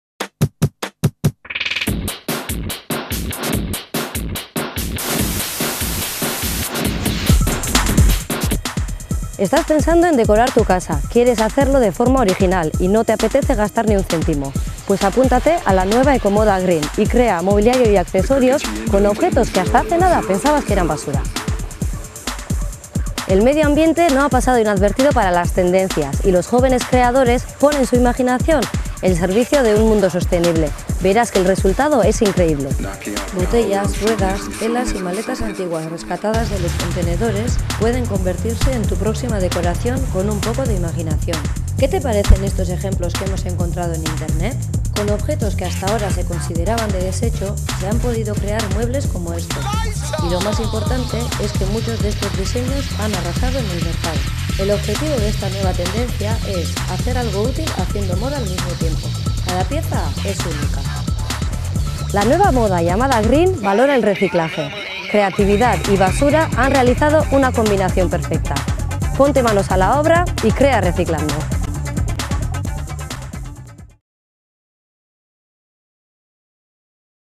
Ecomoda - Gaztebizz
Decora tu casa reciclando y reutilizando.

*Este video, muchos más y toda la información que te interesa